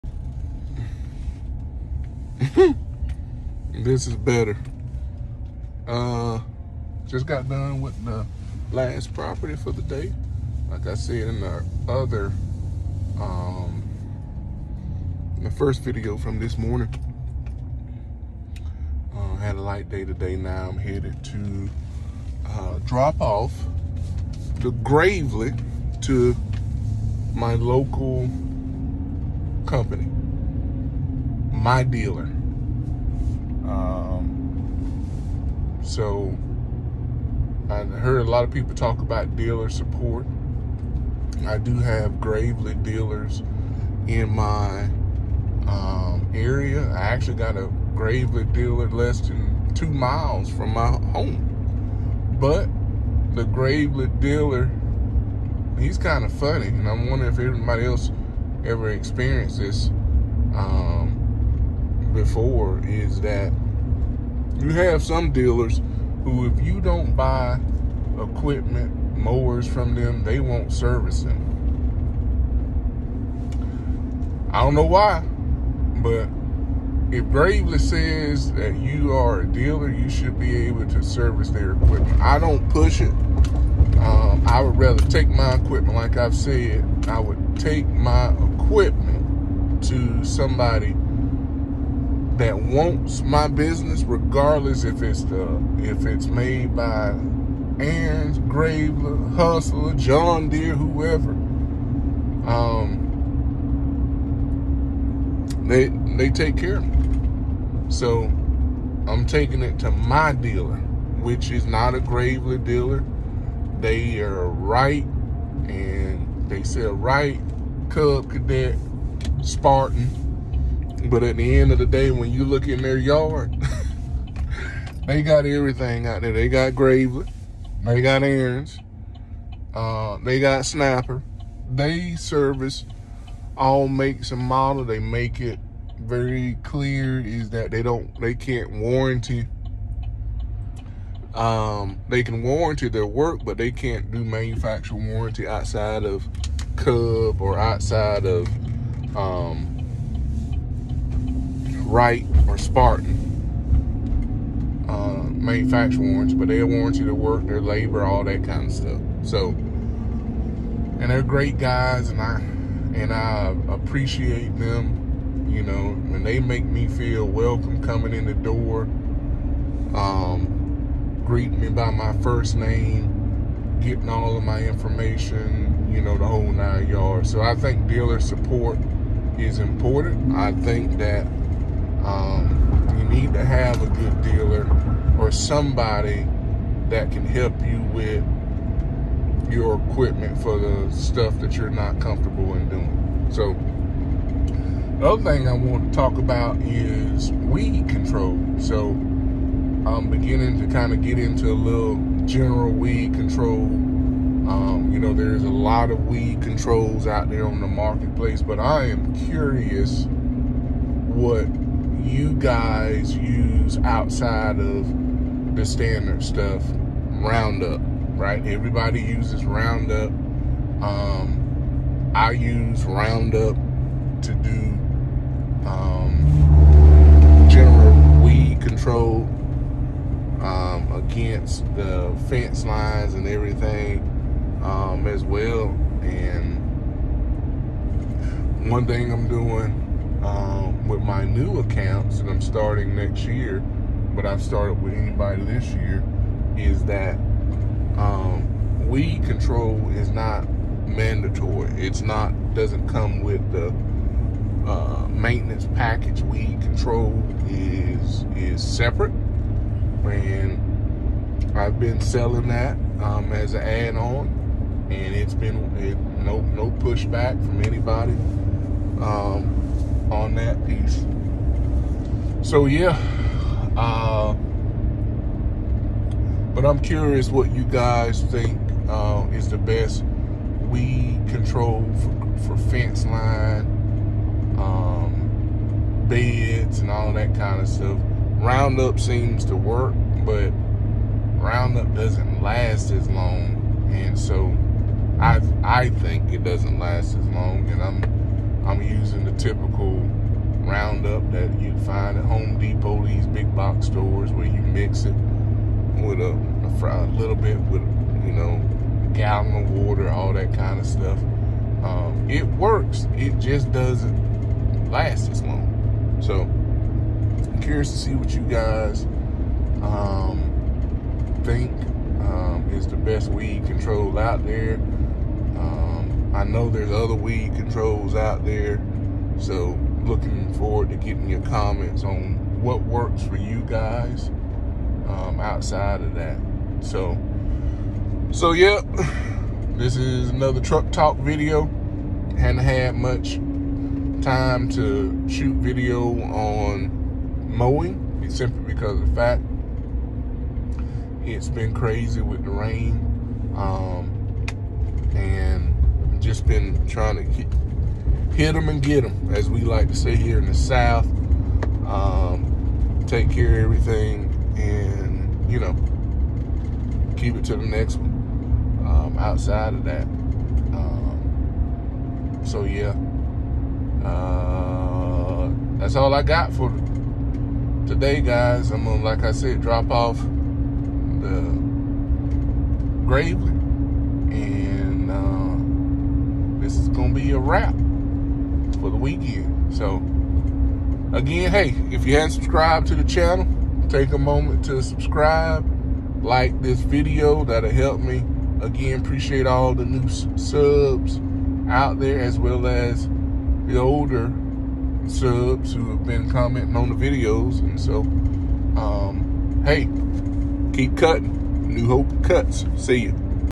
Dealer Support | Help When You Need It | Rain and Mushrooms
Truck talk about Dealer Support and what mine looks like. Rain makes the grass grow and brings in the mushrooms too